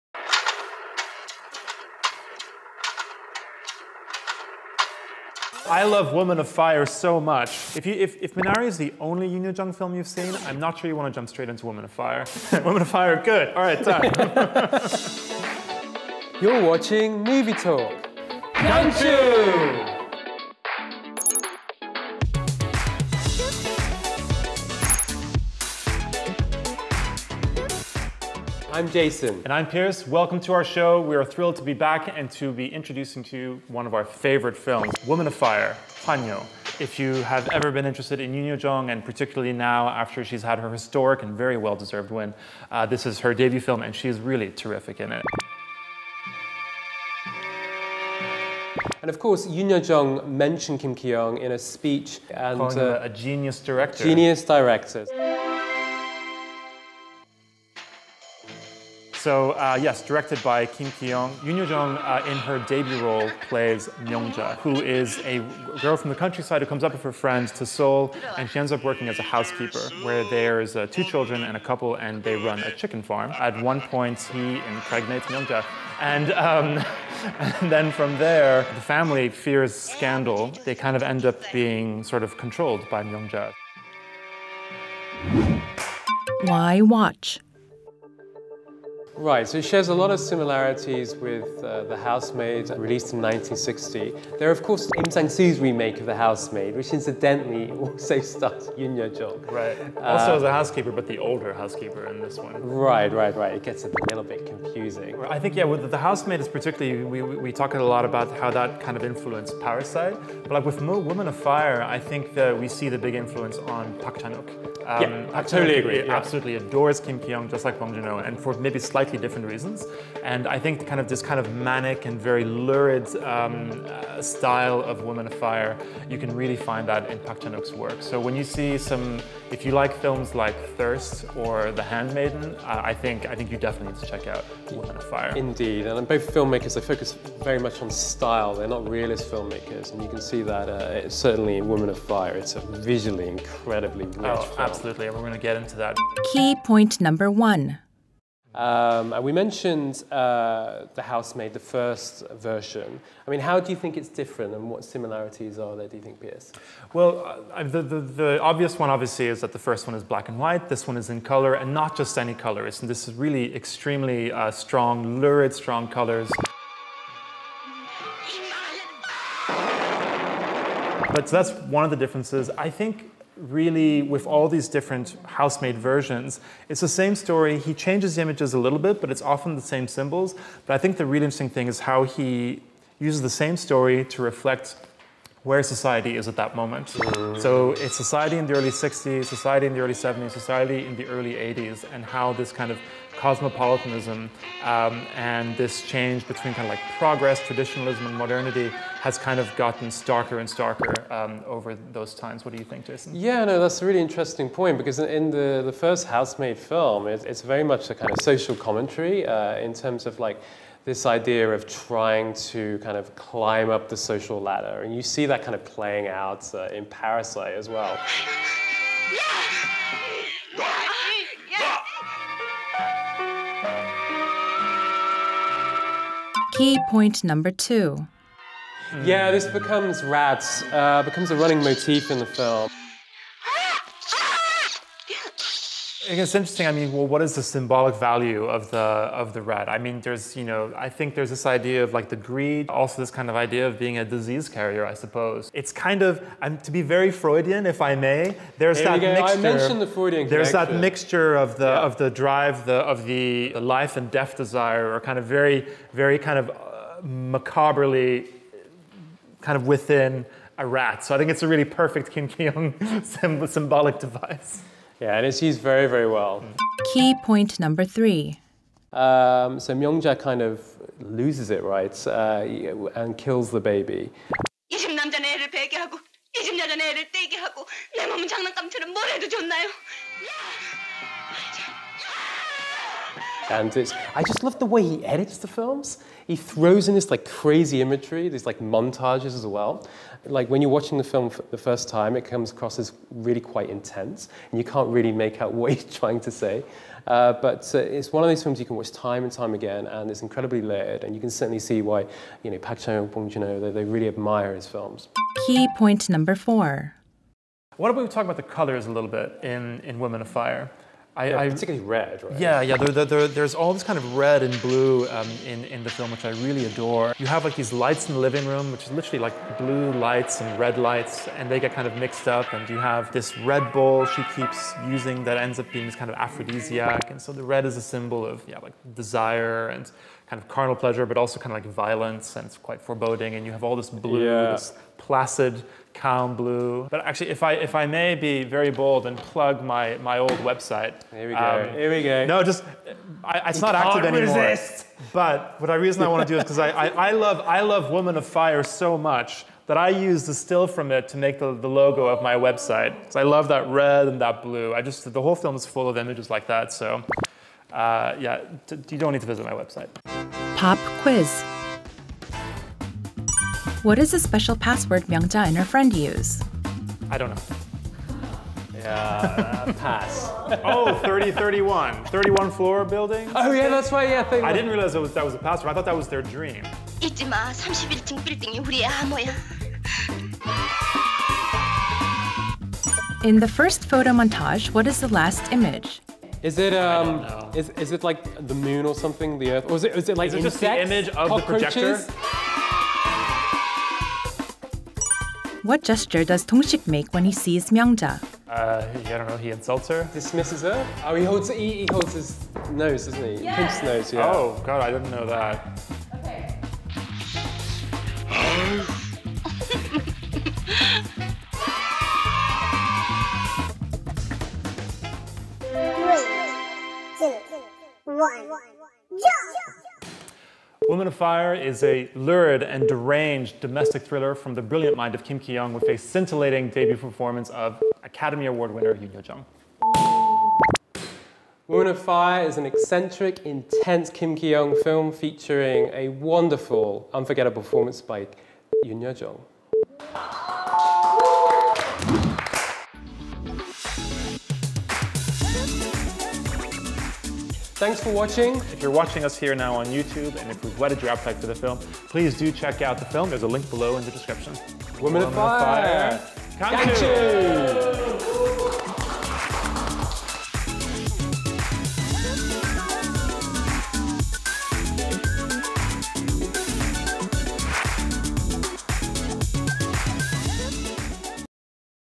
[K-CINEFLEX] Ep21. Youn Yuh-jung's debut film! “Woman of Fire”_MOVIE TALK
[MOVIE TALK] WOMAN OF FIRE

K-Film experts (Jason Bechervaise & Pierce Conran) talk about Korean movies and share their love for Korean cinema with trivia, personal anecdotes and insight for fans worldwide.
Oscar-winning Youn Yuh-jung’s debut film and breakout role - “Woman of Fire(1971)"! “Minari” won her an Oscar. But it was “Woman of Fire” that won her early super rookie status.